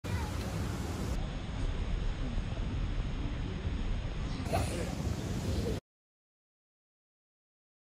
How To Do A Synchronize Cartwheel For Kids
How To Do A Synchronize Cartwheel For Kids, learning a synchronize cartwheel while enjoying the grass in the Flamingo Hotel garden.